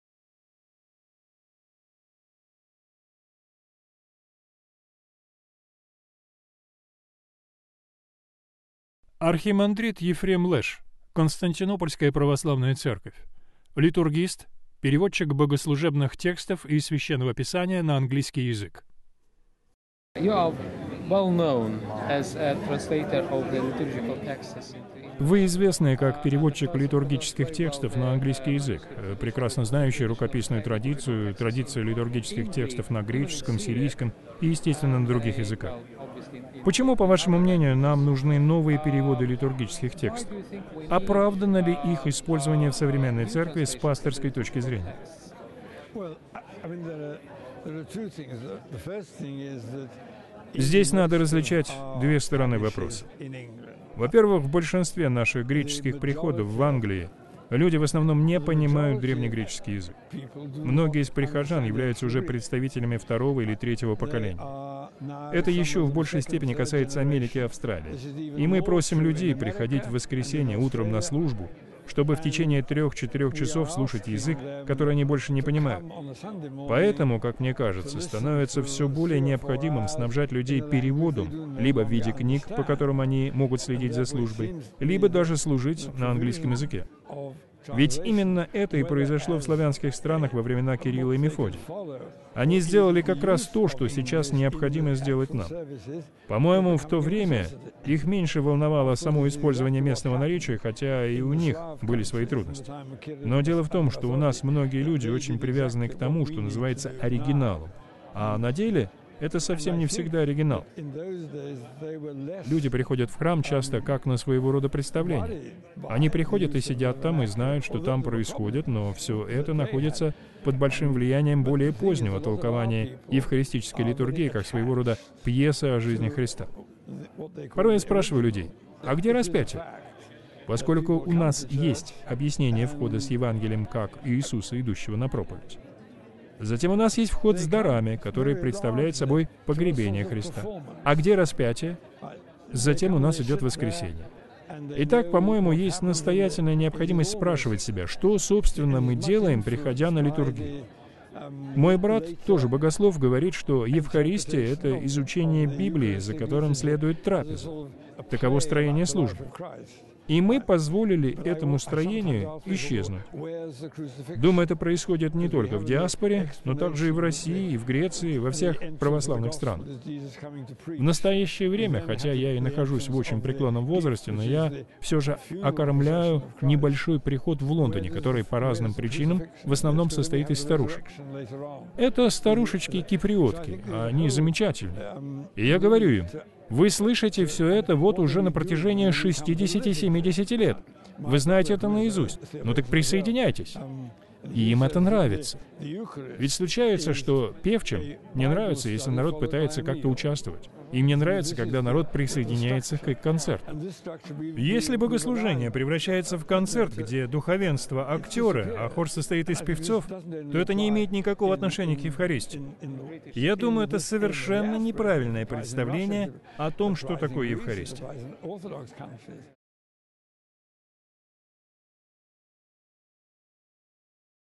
Вопросы перевода богослужения на современный язык
Интервью с известным переводчиком богослужебных текстов на английский язык архимандритом Ефремом (Лэш) -- о значении литургии, неприемлемости символического толкования богослужения и о деле святых Кирилла и Мефодия.